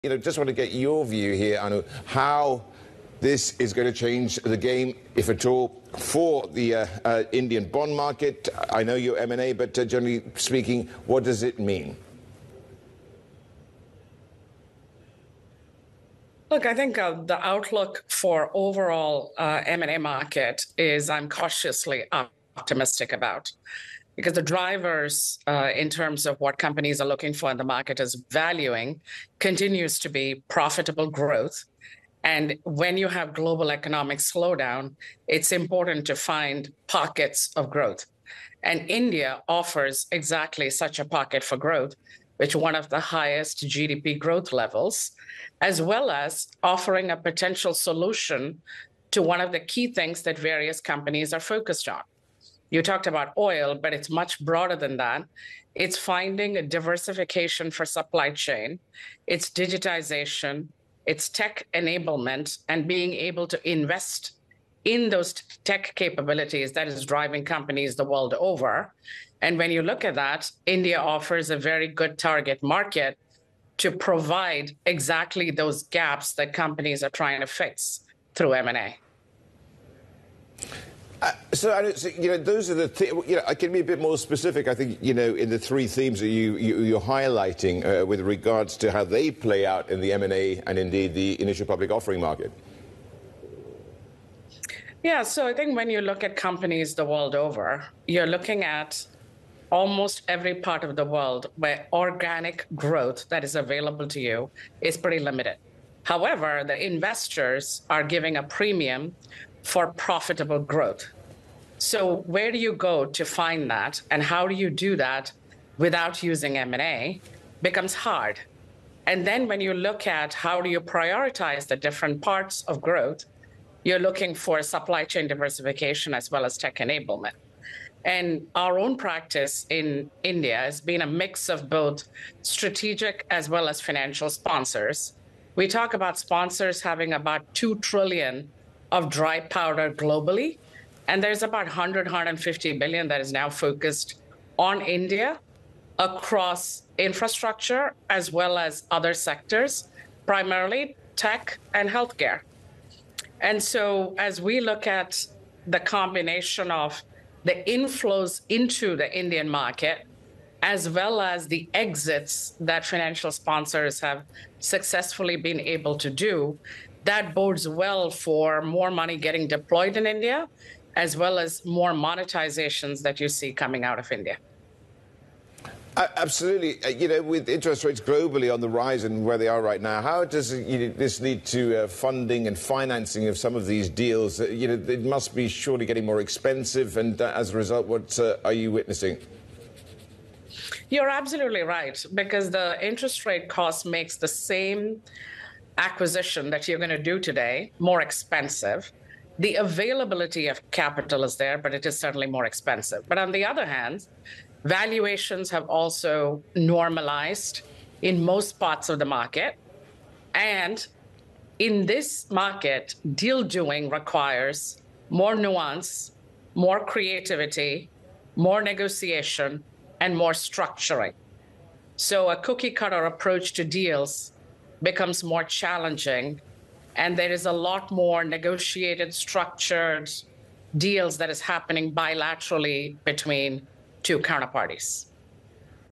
JPMorgan’s Aiyengar Cautiously Optimistic on M&A Market
Anu Aiyengar, managing director and global head of mergers and acquisitions at JPMorgan, discusses the M&A market, where she’s finding pockets of opportunity and her outlook for the Indian market. She speaks on Bloomberg Television.